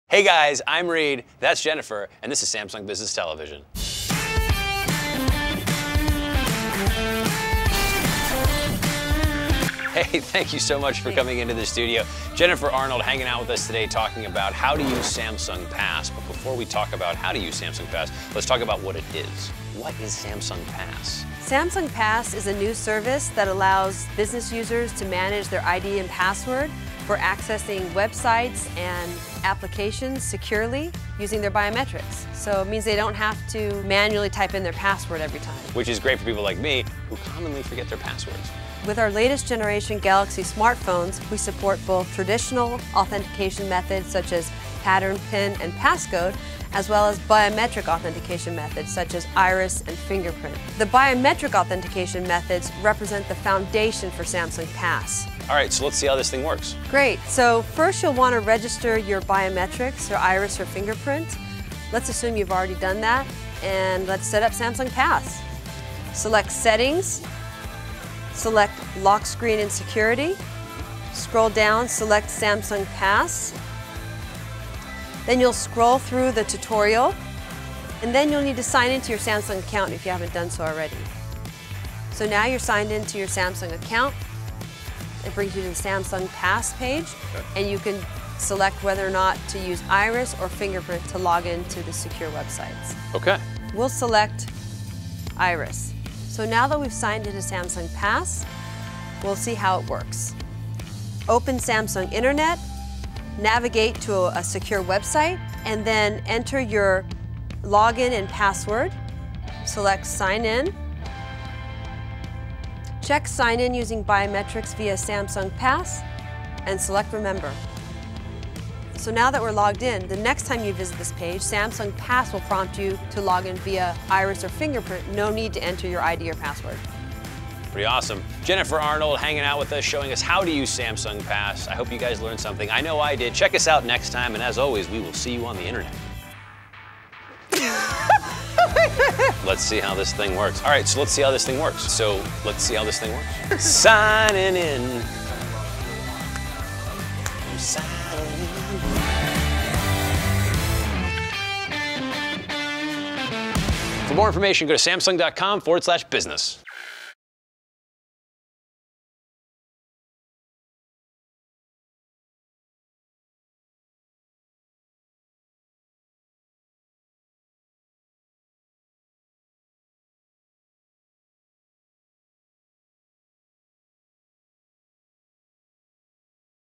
How to Use Samsung Pass | SBTV
Log-ins and passwords are clunky and don't always guarantee security. In this episode, Reid Estreicher and Jennifer Arnold demonstrate how Samsung Pass can use iris and fingerprint scans to secure mobile devices more easily and effectively.